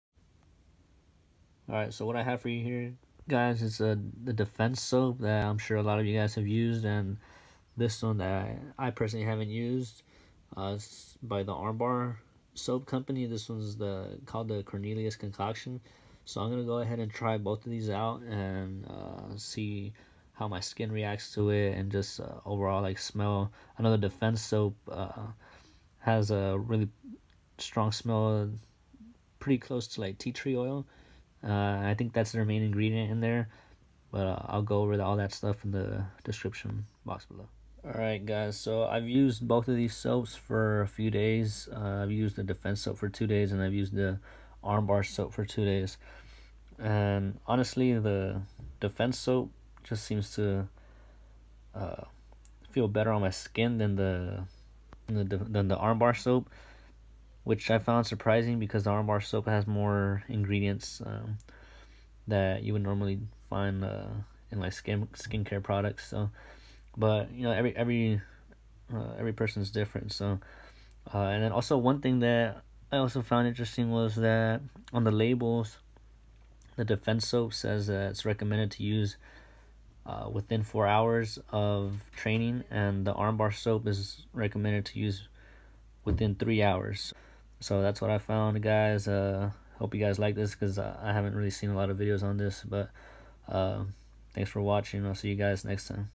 Defense soap and armbar soap
A look at the two most popular hygiene brands in the Jiu Jitsu and combat sports community.